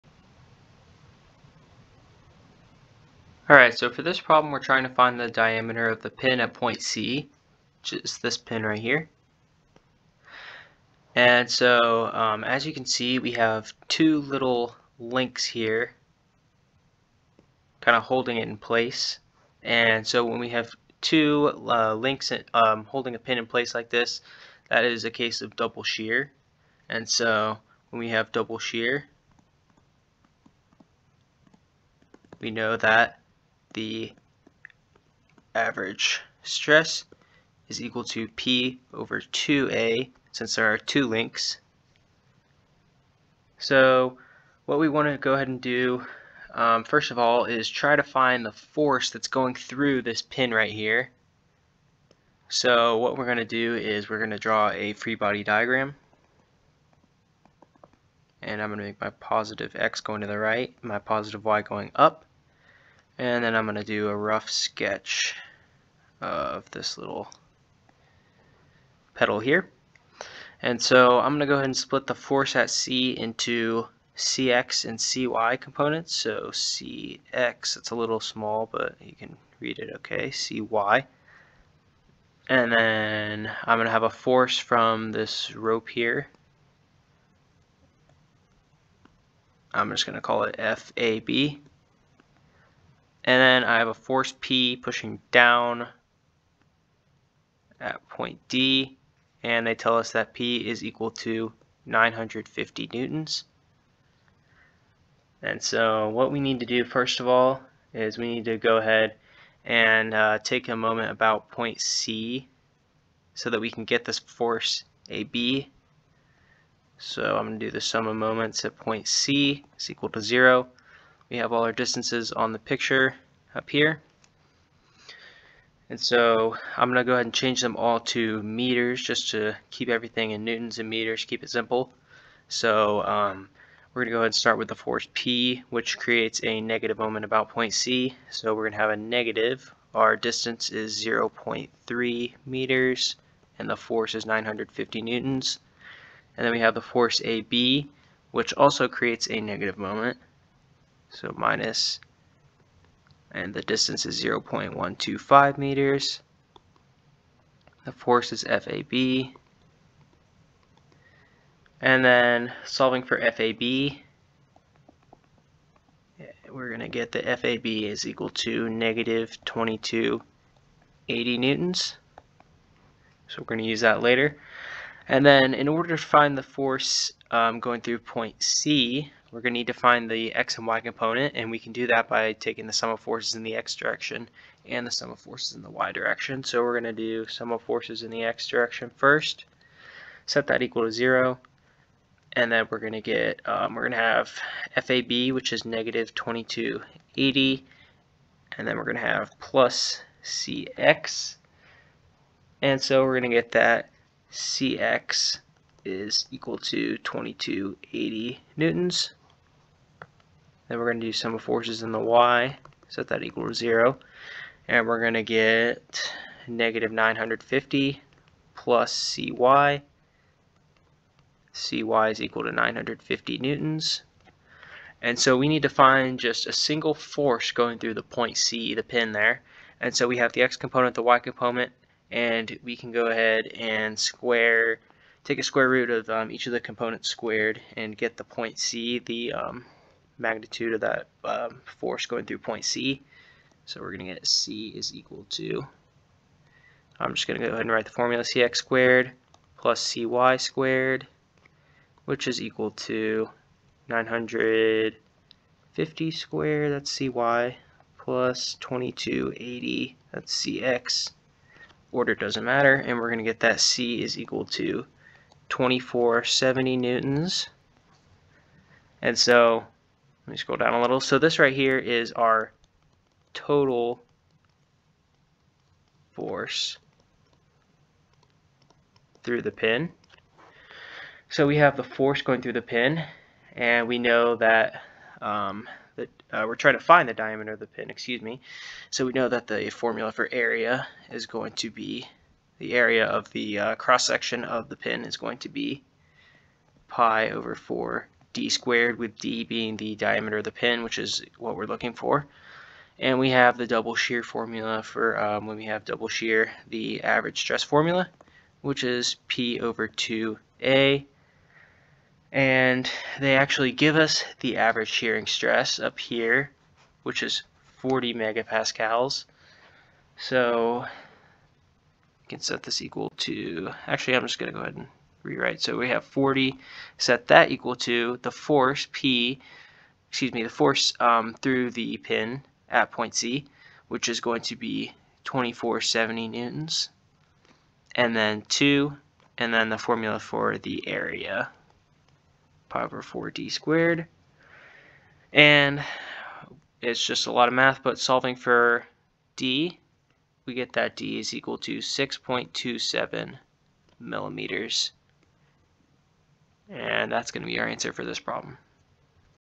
Find the diameter of the pin at C
A force P of magnitude 950 N is applied to the pedal shown.

NOTE: This is a multi-part question. Once an answer is submitted, you will be unable to return to this part.

Determine the diameter of the pin at C for which the average shearing stress in the pin is 40 MPa.